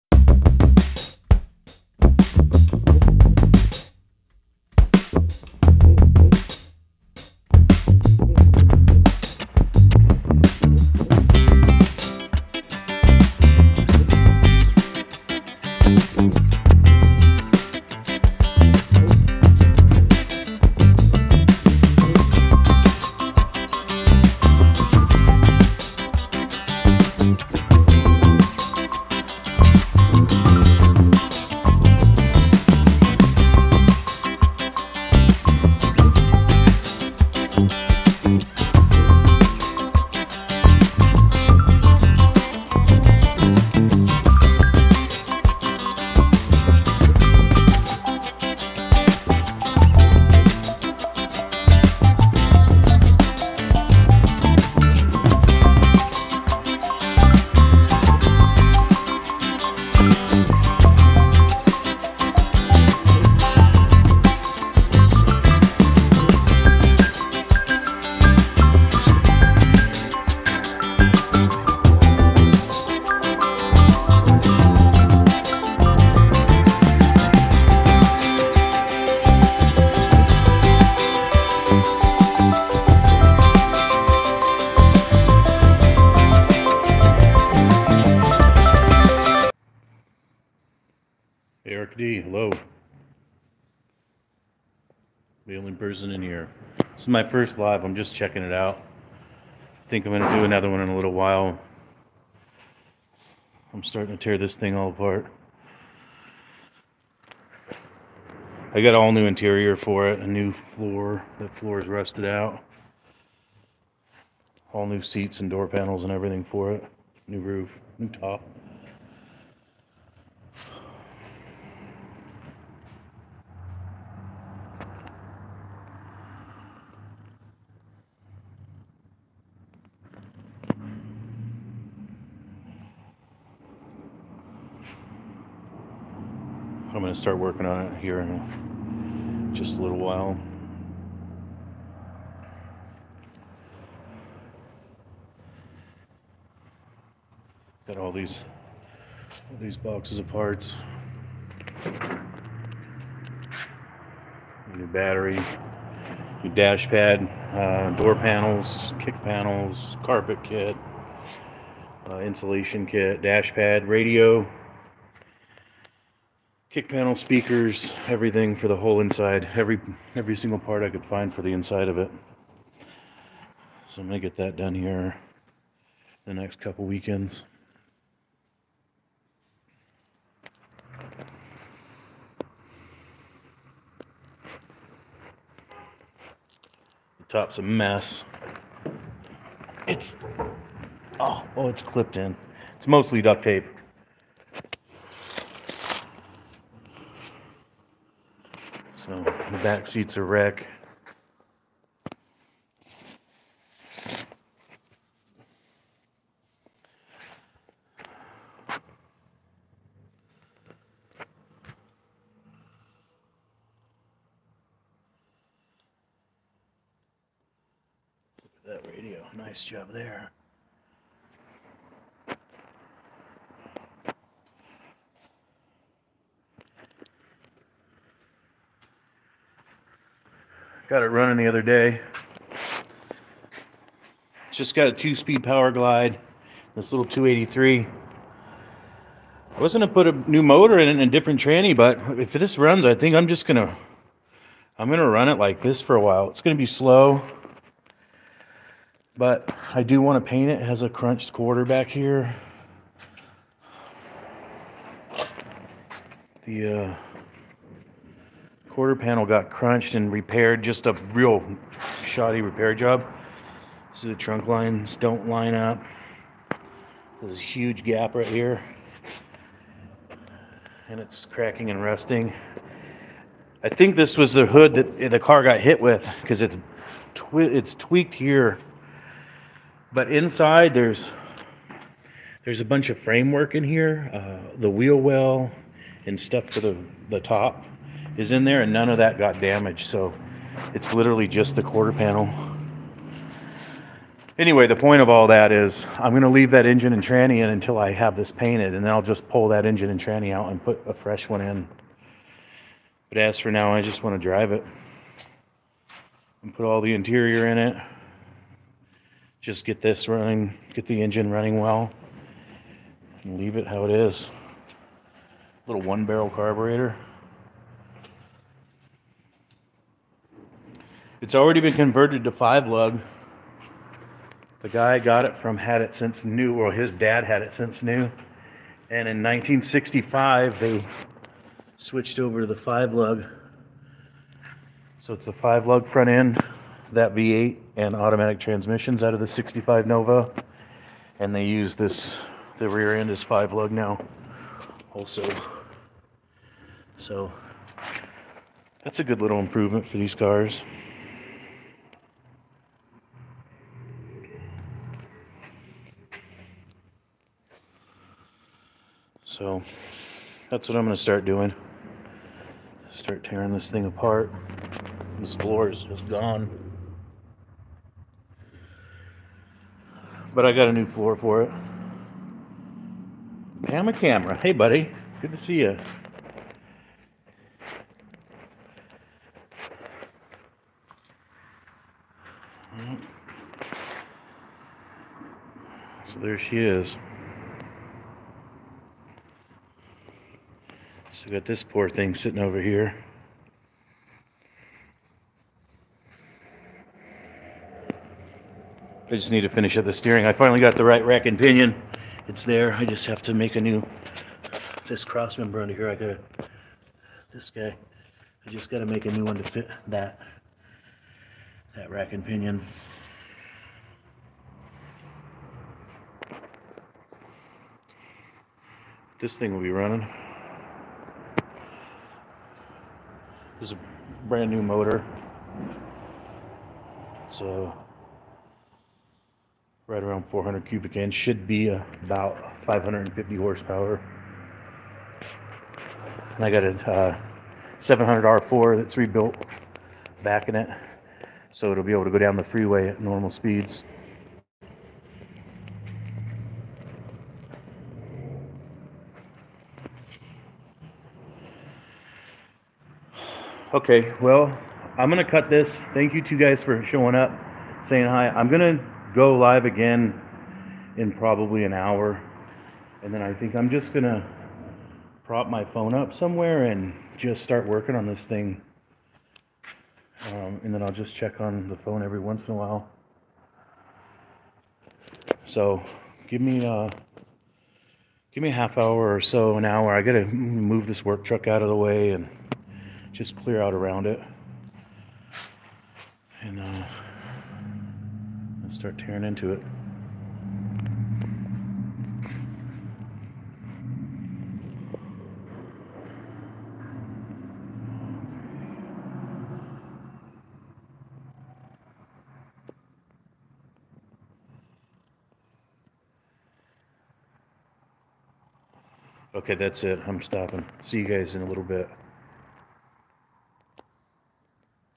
1963 Chevy ll Nova Convertible floor replacement
I am working on welding in replacement floors in my 1963 Chevy Nova convertible.  I bought a replacement floor from classic industries. I cut out the old rust and made a clean opening to replace the floor.